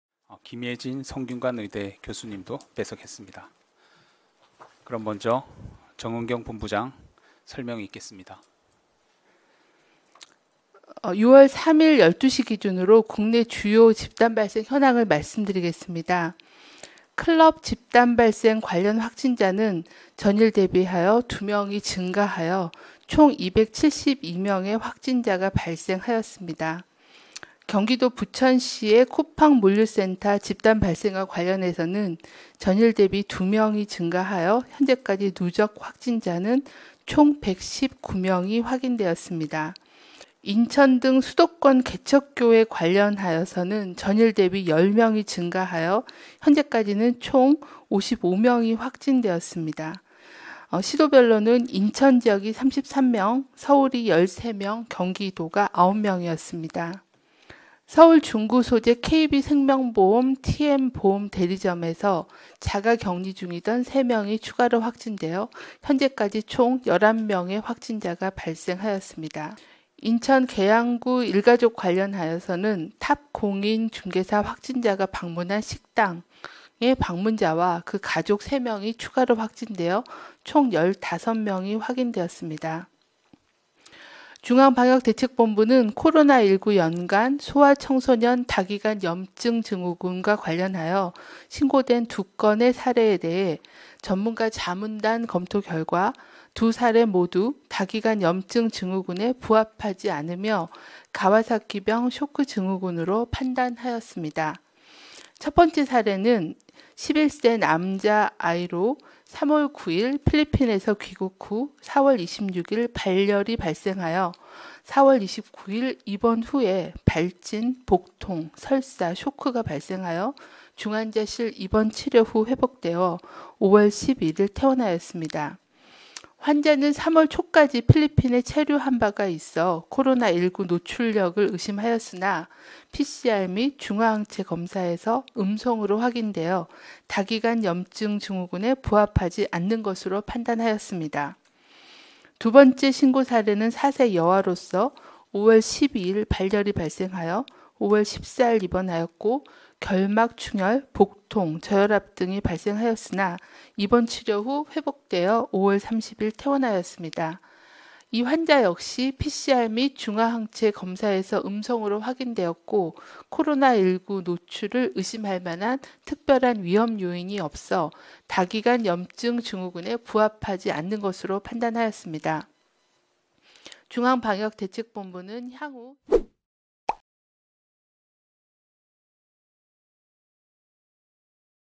수도권 교회 집단감염 초비상…인천 개척교회 관련 최소 55명 / 연합뉴스 (Yonhapnews)
(서울=연합뉴스) 수도권 교회 소모임 집단감염 여파로 신종 코로나바이러스 감염증(코로나19) 확진자가 하루가 다르게 늘어나고 있습니다.
중앙방역대책본부(방대본)는 3일 정오 기준으로 인천·경기 등 수도권 개척교회와 관련한 코로나19 확진자가 10명 늘어 누적 확진자는 55명이라고 밝혔는데요.
서울 이태원 클럽과 경기 부천의 쿠팡물류센터에서의 집단감염 '여진'이 이어지면서 이태원 클럽 관련 누적 확진자는 272명으로, 쿠팡물류센터 관련 확진자는 119명으로 각각 늘었습니다.국내에서 처음 보고된 소아·청소년 다기관염증증후군, 이른바 '어린이 괴질' 의심 사례 2건의 경우, 이 증후군이 아닌 가와사키병 쇼크증후군으로 판단됐습니다.